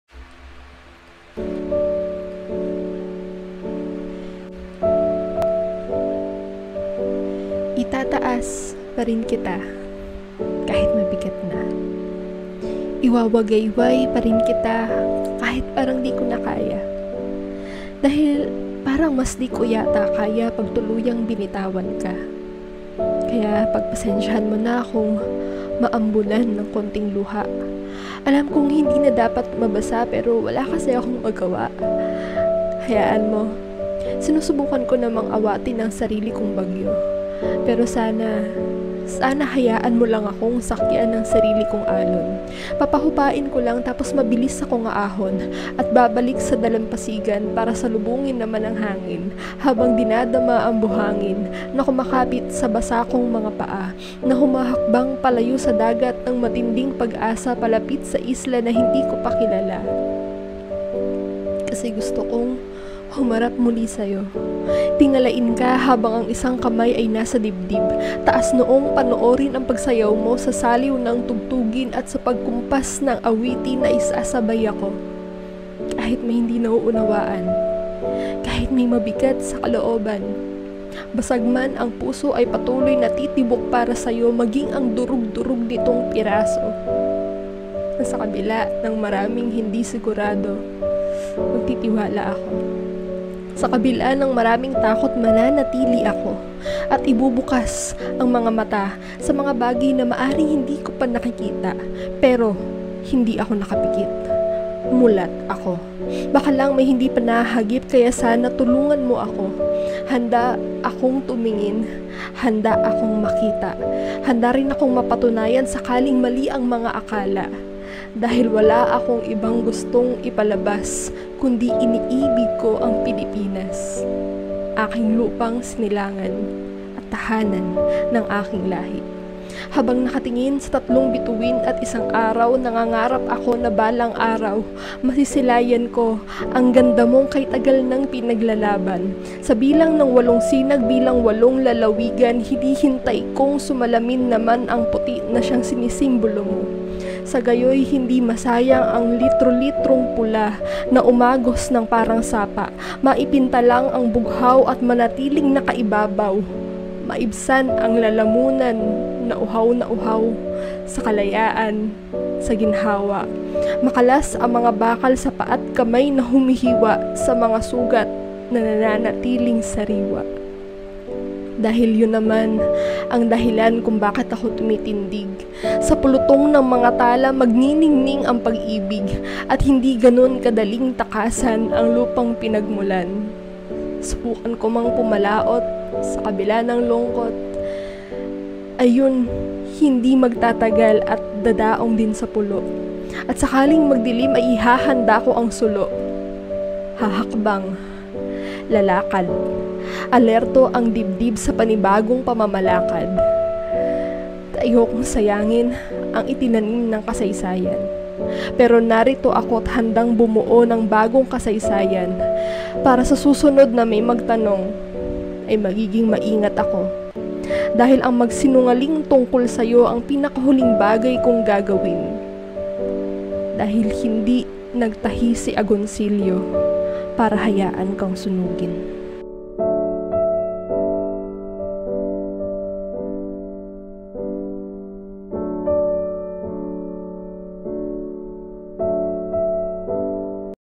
Itataas Pa Rin
With everything that is happening with the country today, may we continue to raise the flag, showing love for the Philippines.

A wonderful piece written and voiced by Jeca Mae Ramis.